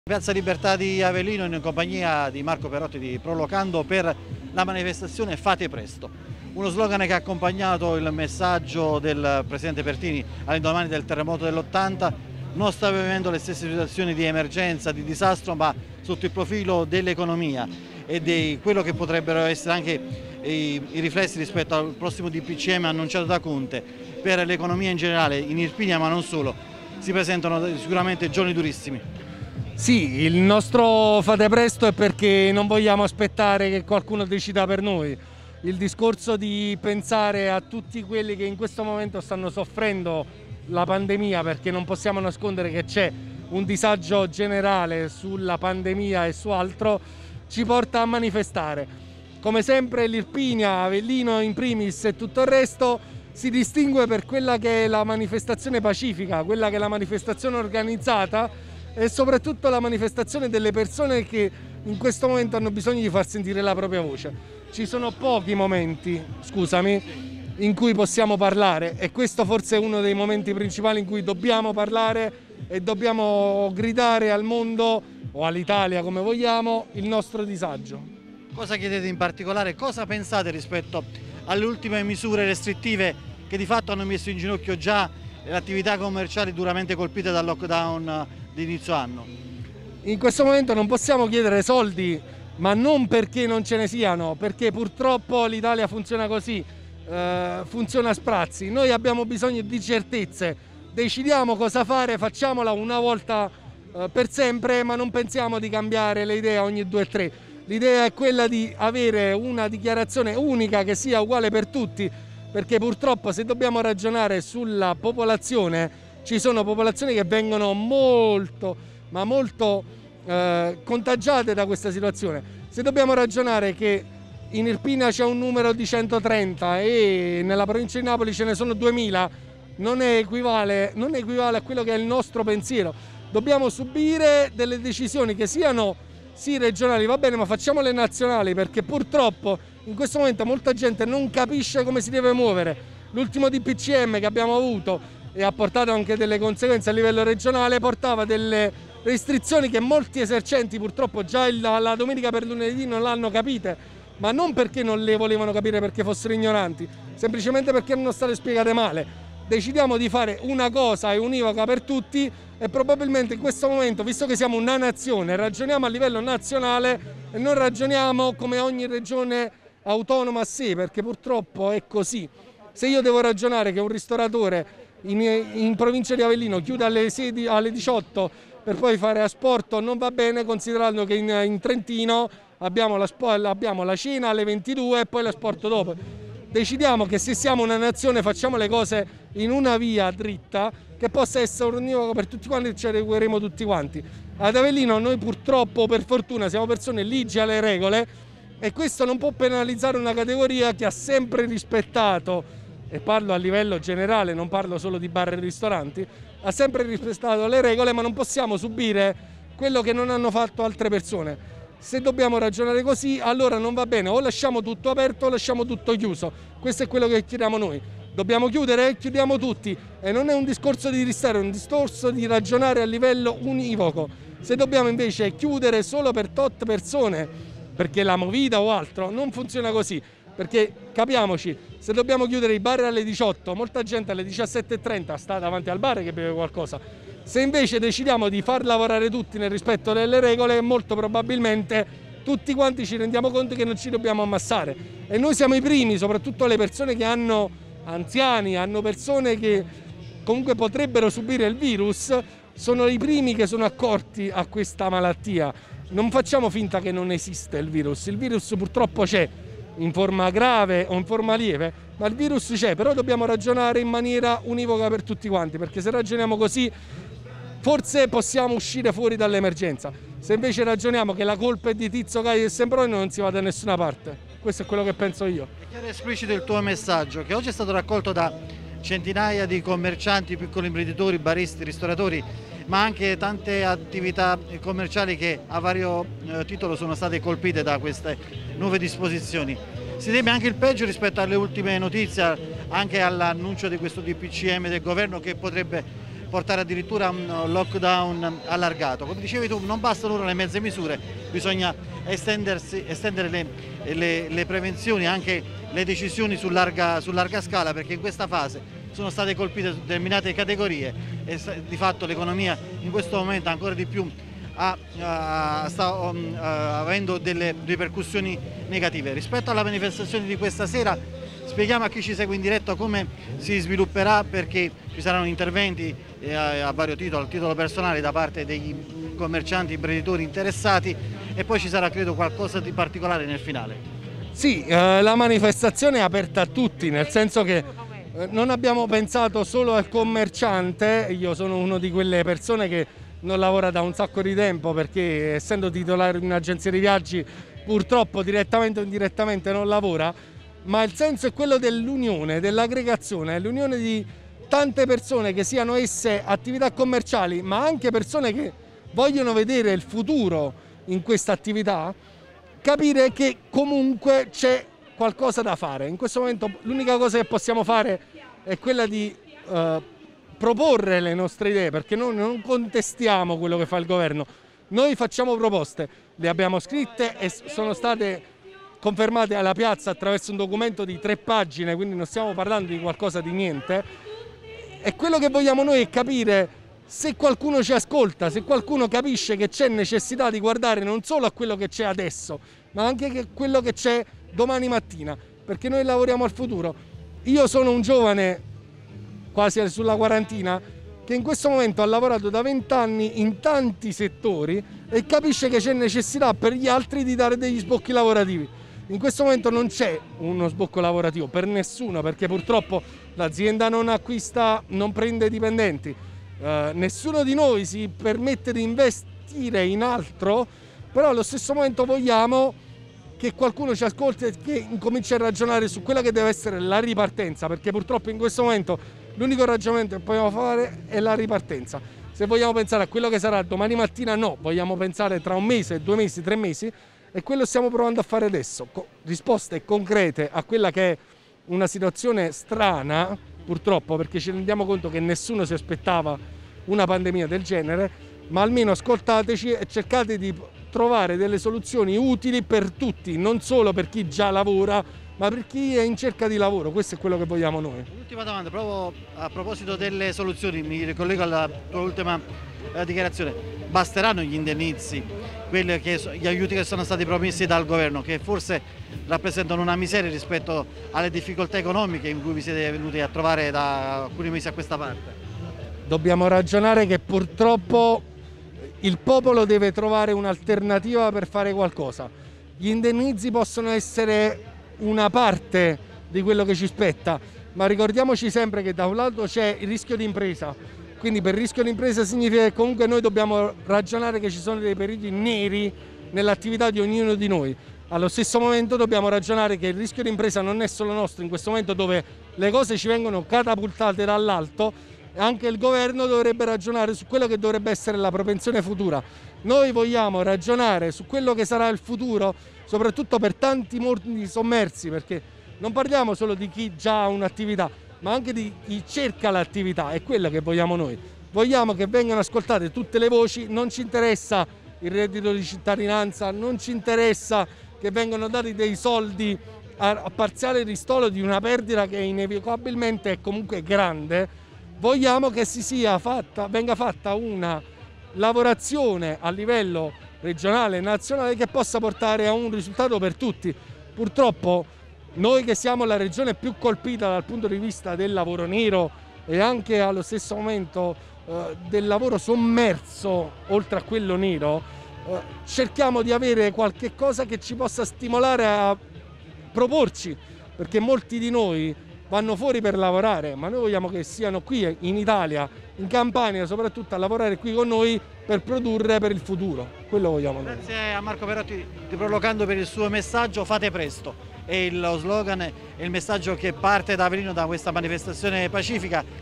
"Fate presto", l'Irpinia scende in piazza
Manifestazione pacifica di commercianti, imprenditori ed associazioni ad Avellino per manifestare il malessere per le misure adottate per combattere la pandemia.